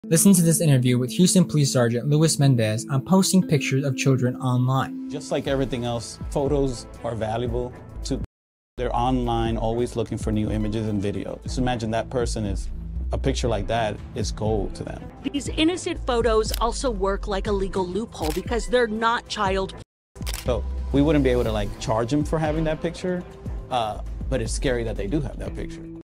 The Risk of Posting Your Child's Photo Online!!!
A Huston Police Sargent talks about the risk of Posting photos of children online!